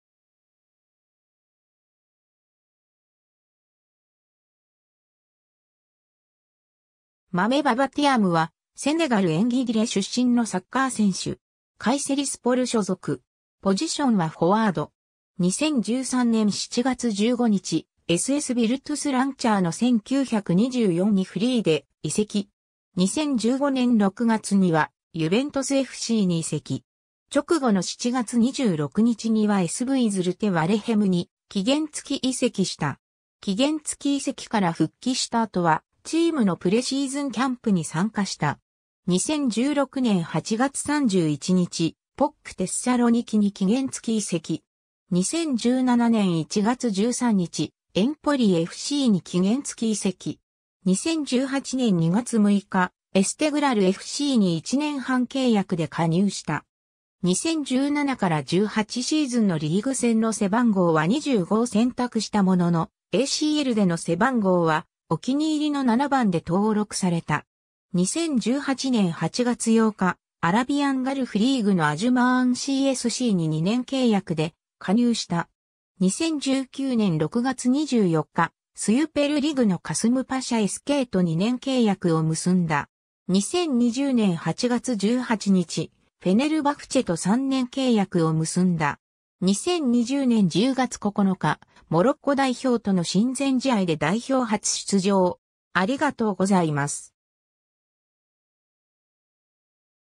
マメ・ババ・ティアム
SSヴィルトゥス・ランチャーノ1924の選手
SVズルテ・ワレヘムの選手
マメ・ババ・ティアム（Mame Baba Thiam、1992年10月9日 - ）は、セネガル・エンギディレ出身のサッカー選手。
カイセリスポル所属。
 2013年7月15日、SSヴィルトゥス・ランチャーノ1924にフリーで移籍。
 2015年6月にはユヴェントスFCに移籍。
直後の7月26日にはSVズルテ・ワレヘムに期限付き移籍した。
期限付き移籍から復帰した後はチームのプレシーズンキャンプに参加した。
 2016年8月31日、PAOKテッサロニキに期限付き移籍。
 2017年1月13日、エンポリFCに期限付き移籍。
 2018年2月6日、エステグラルFCに1年半契約で加入した。
2017-18シーズンのリーグ戦の背番号は25を選択したものの、ACLでの背番号はお気に入りの7番で登録された。
 2018年8月8日、アラビアン・ガルフ・リーグのアジュマーンCSCに2年契約で加入した。
 2019年6月24日、スュペル・リグのカスムパシャSKと2年契約を結んだ。
 2020年8月18日、フェネルバフチェと3年契約を結んだ。
 2020年10月9日、モロッコ代表との親善試合で代表初出場。